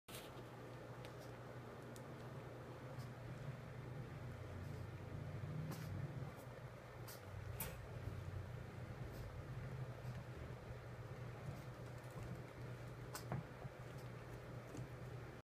Rabbit Mauling
Little Mikey is a 3lb, 10oz Applehead Chihuahua rescue from Taos, NM.   He now lives in Colorado with his new family.